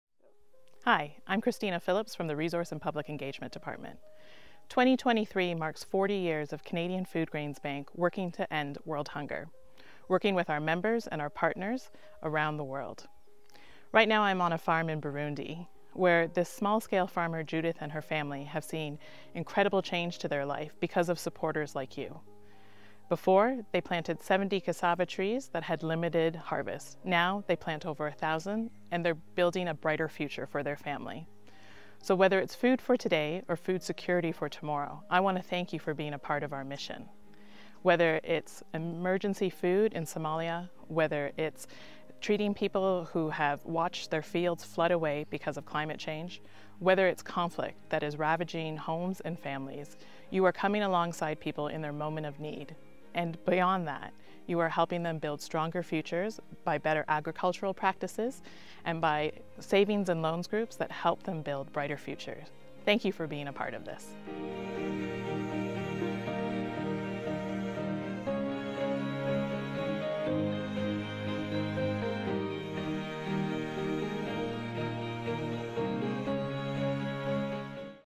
Thank you for supporting us in 2023!
Our director of resources and public engagement Christina Philips shares a message of thanks from the thriving field of a cassava farmer in Burundi where after many years of conflict, the country is using this time of peace to rebuild.